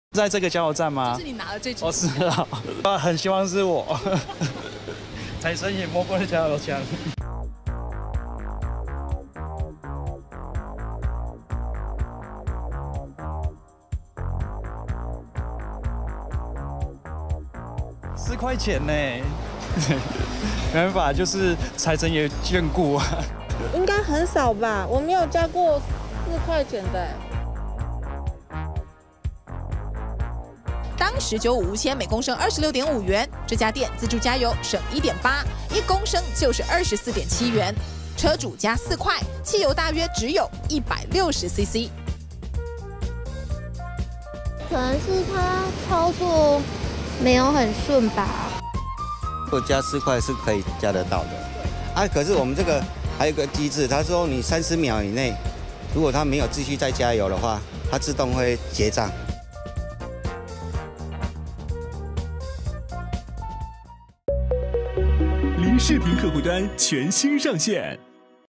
他加了3两多汽油，获两百多万巨奖
台湾财政部门近日公布今年1月和2月发票千万特别奖得主。一人仅花4元新台币（不到人民币1元）加了3两多油，获此大奖——约合人民币226万元。